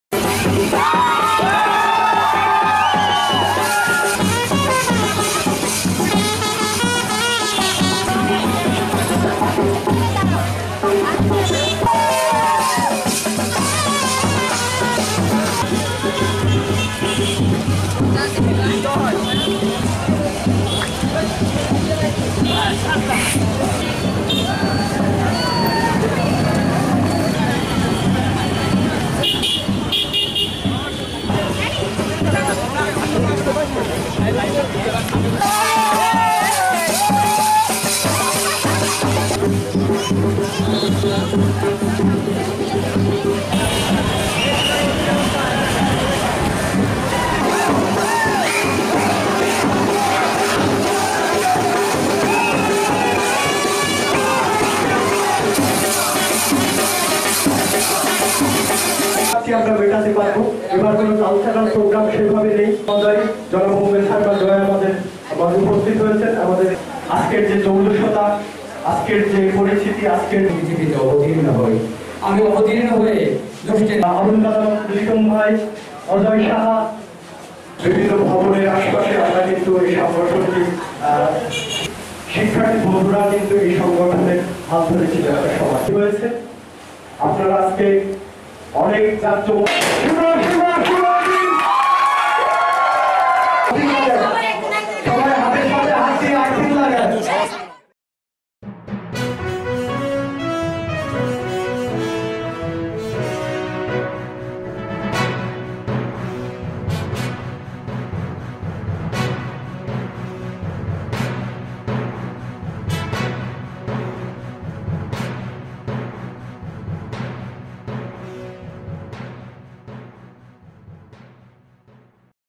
ঝিনাইদহে কসাসের ১ যুগ পদার্পন উপলক্ষে বর্ণাঢ্য আয়োজন
১ যুগে পদার্পন করলো জয়বাংলা ইয়ুথ এ্যাওয়ার্ড প্রাপ্ত সংগঠন ঝিনাইদহের কথন সাংস্কৃতিক সংসদ (কসাস)। এ উপলক্ষে আজ সকালে সরকারি কেসি কলেজ চত্বর থেকে একটি শোভাযাত্রা বের করা হয়। শোভাযাত্রাটি শহরের বিভিন্ন সড়ক ঘুরে একই স্থানে এসে শেষ হয়।